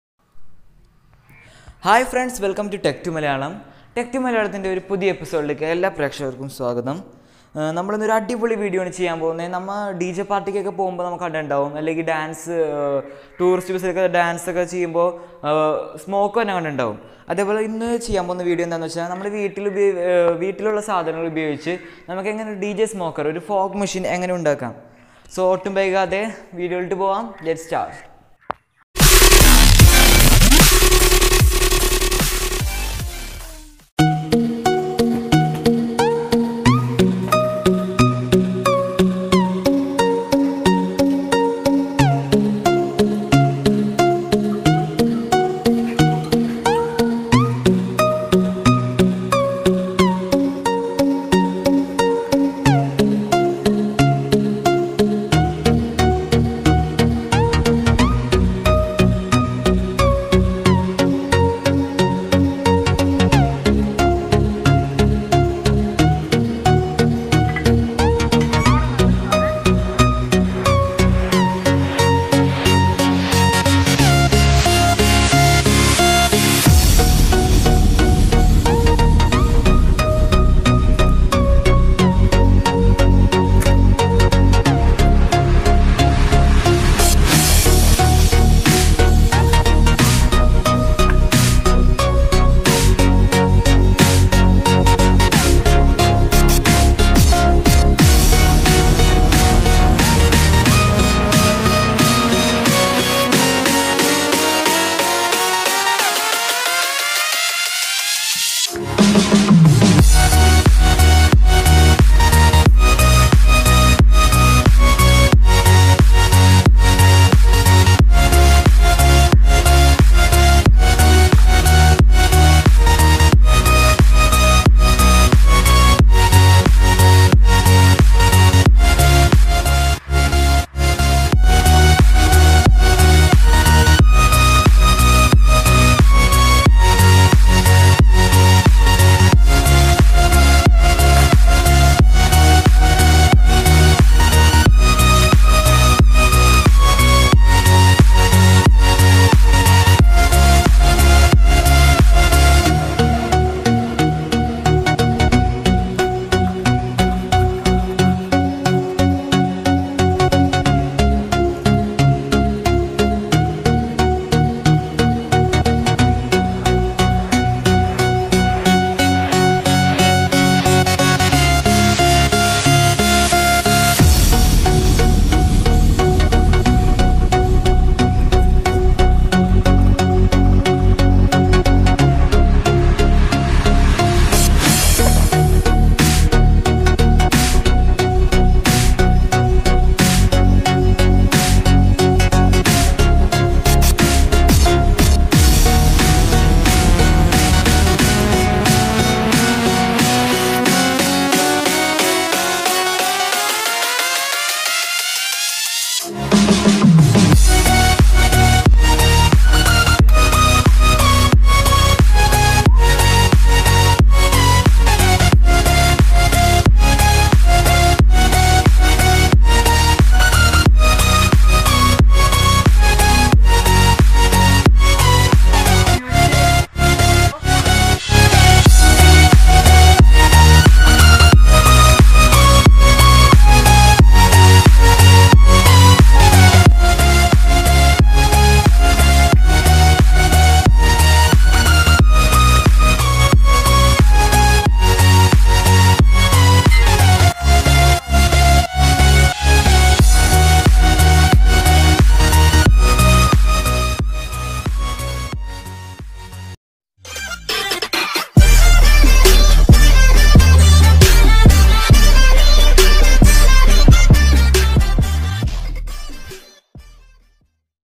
How to Make Fog Machine at Home
Items
box
pipe
cpu fan